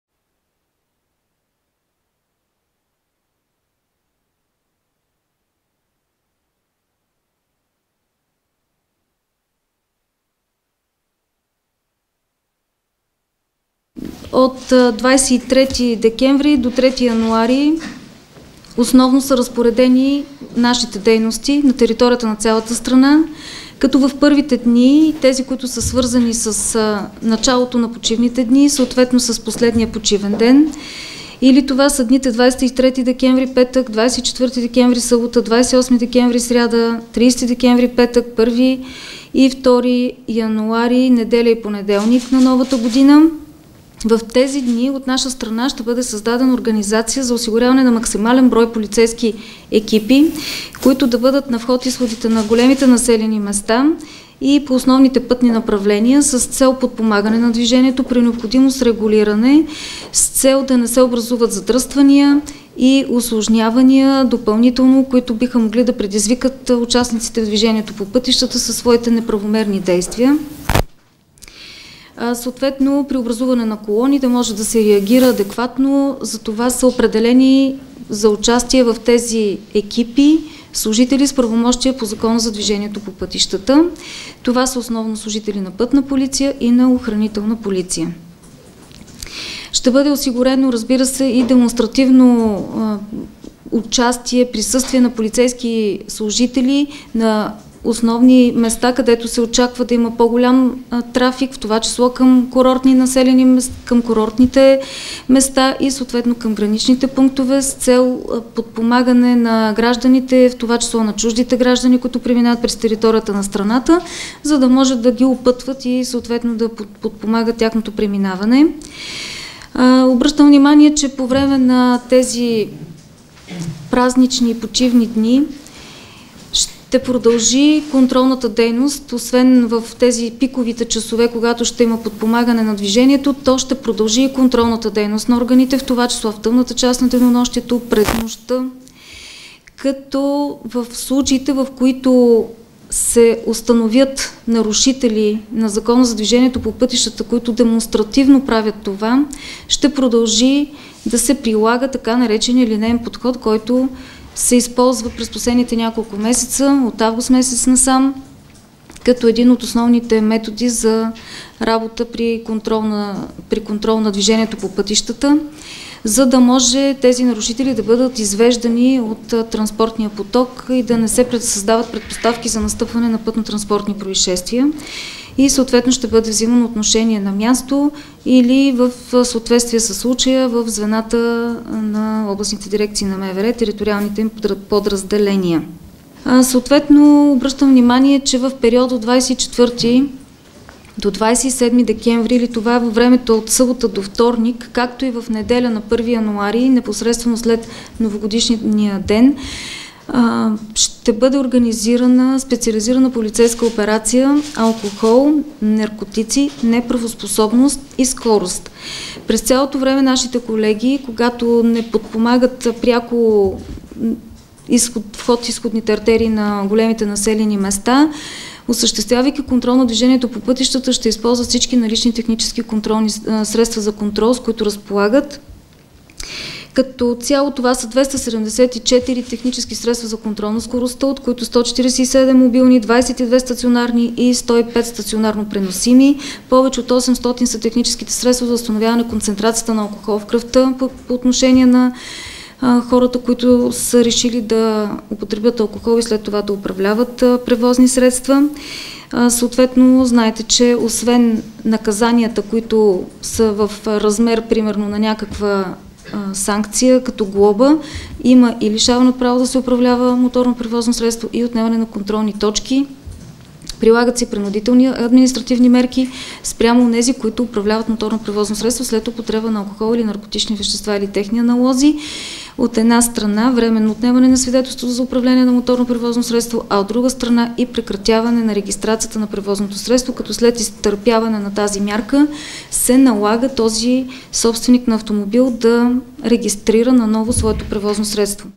Полиция на пътя - масиране проверки преди и по време на празниците
От МВР дадоха и съвети за това как да не станем жертва на престъпления по време на предстоящите почивни дни.